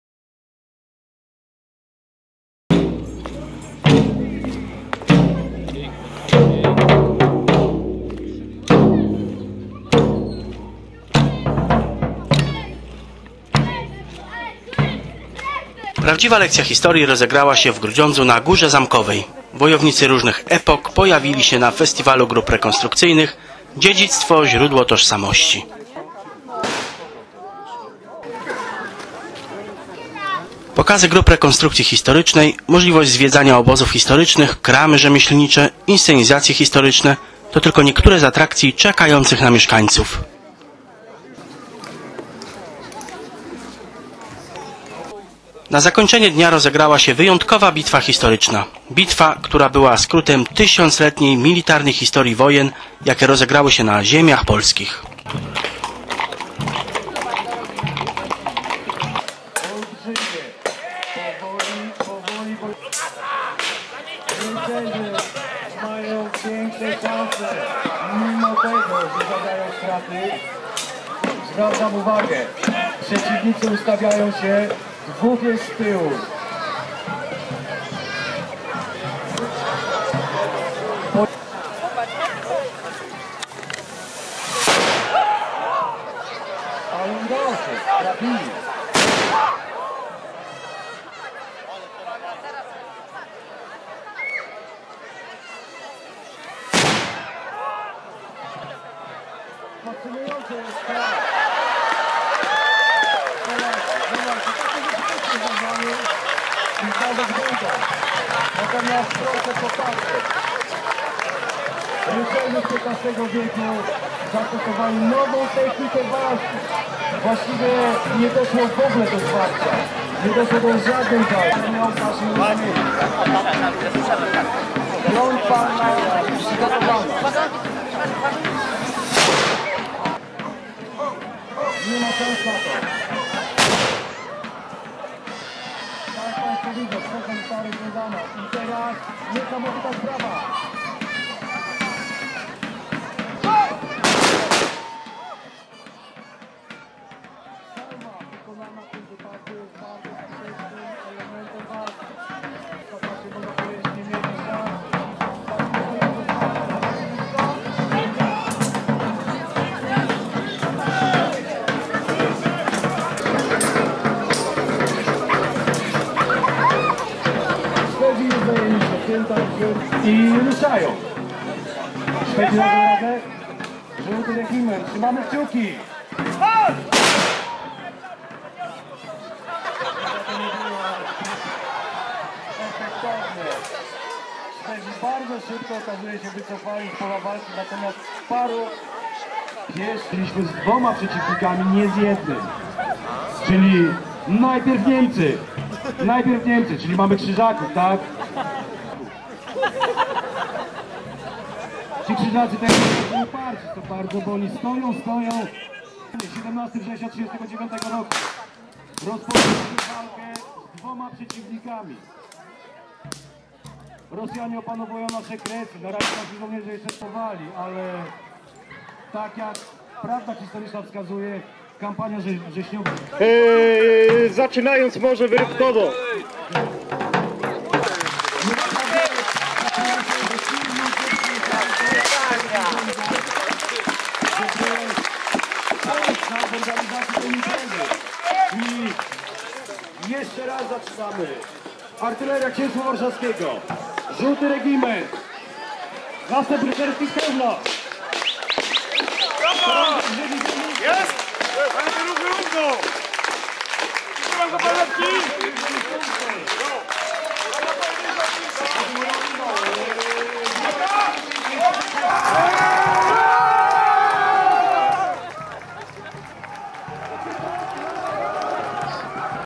Festiwal grup rekonstrukcyjnych na Górze Zamkowej
Prawdziwa lekcja historii rozegrała się w Grudziądzu na Górze Zamkowej. Wojownicy różnych epok pojawili się na Festiwalu „Dziedzictwo - źródło tożsamości”. Organizatorami imprezy były Urząd Miejski i Gildia Działań Historycznych „Alte Knochen”